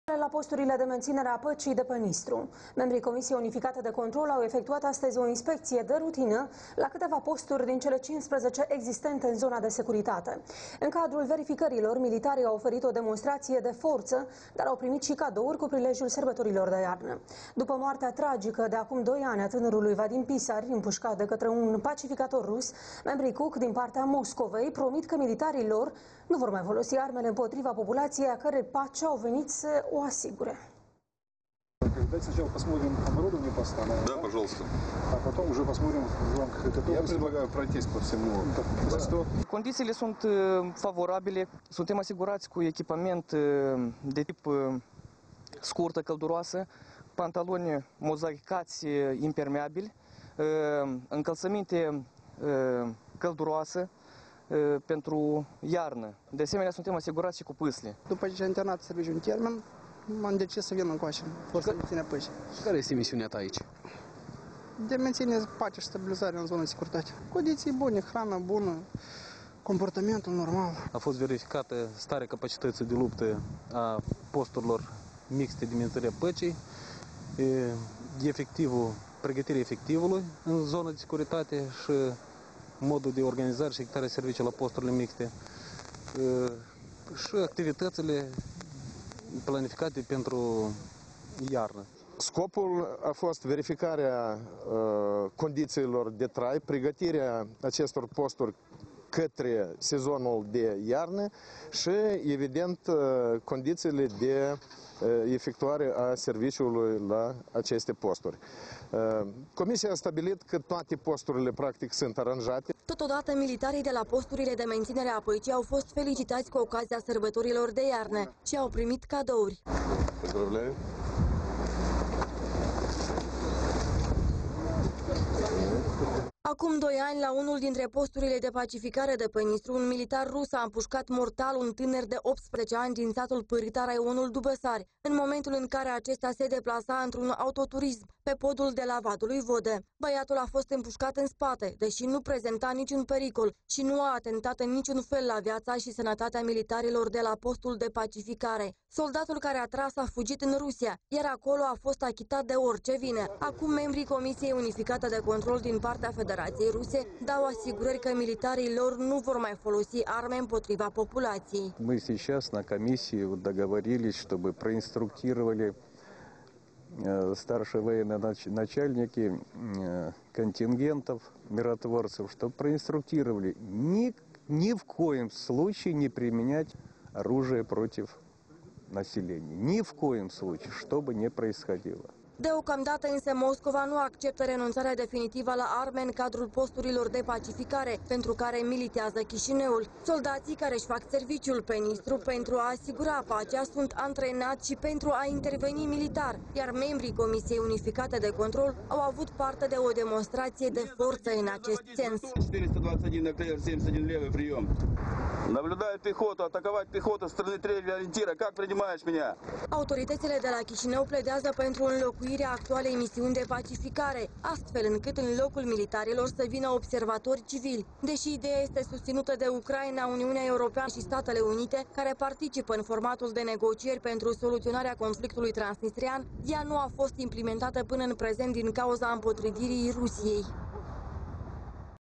На миротворческих постах на Днестре прошли проверки
Вчера члены Объединенной контрольной комиссии провели плановую проверку на нескольких из 15 существующих постах в зоне безопасности.